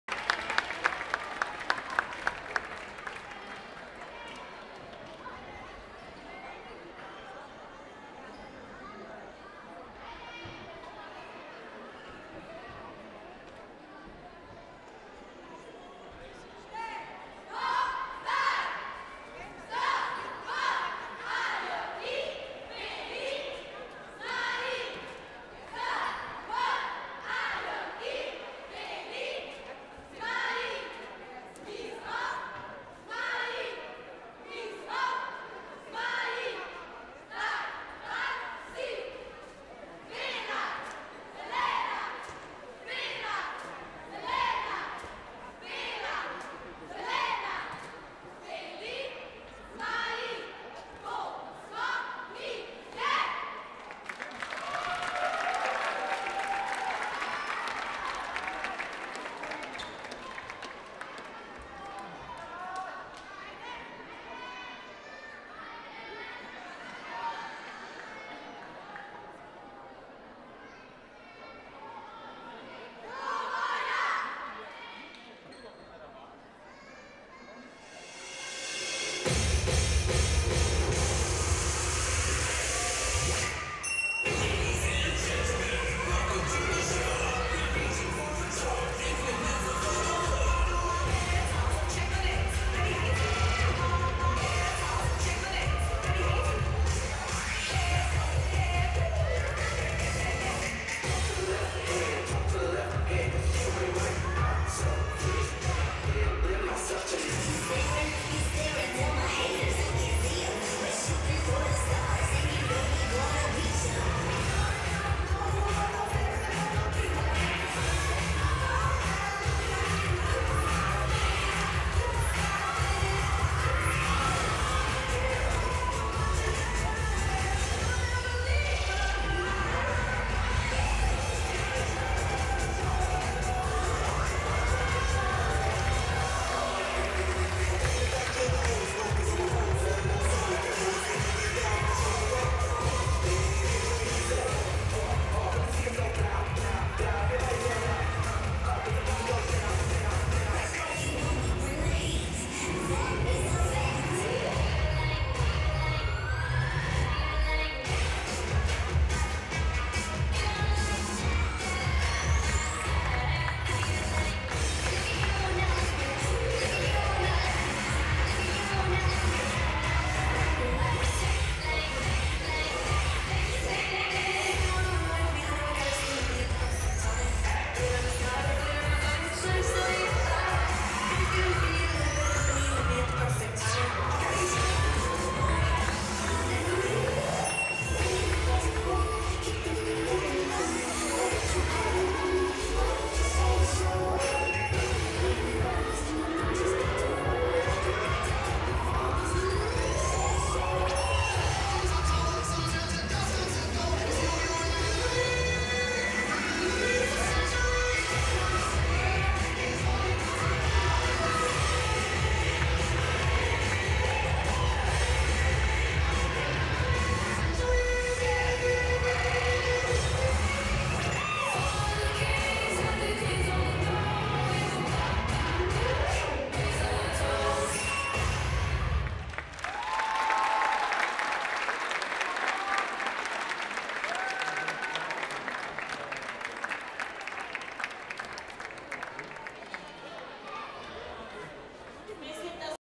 TO 2022 — JTCLA — Beli Zmaji {ŠD GIB Ljubljana Šiška — SVN}
Twist Open 2022 – 21st of May 2022 (Nova Gorica, Slovenia)
International cheerleading and performance cheer competition, part of European Cheer League